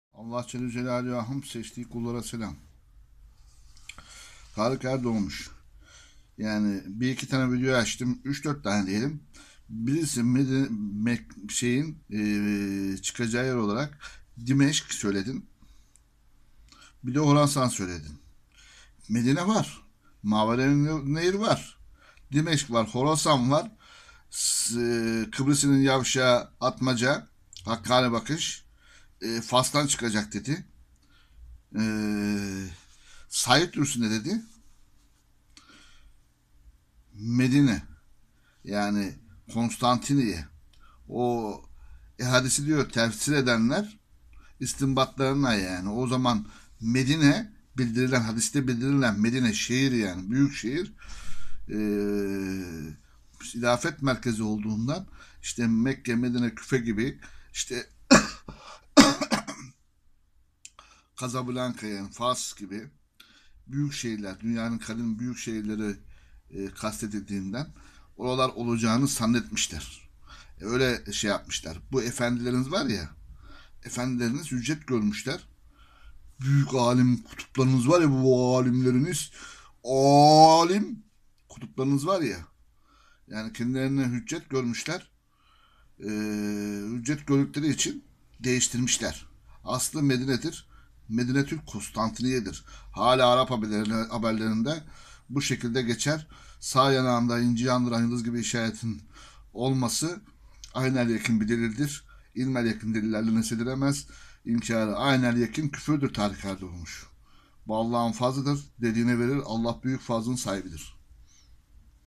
mehdi maveraünnehir, şam ,dımeşk, fas,horosan,medine den çıkacak,, hangisi? tarık erdoğmuş?
zamanın alim geçinenlerin sistemleri efendileri nin dinlerine çağırıyor ,, ben  RESUL ALLAH s.a.v in hak dinine çağırıyorum.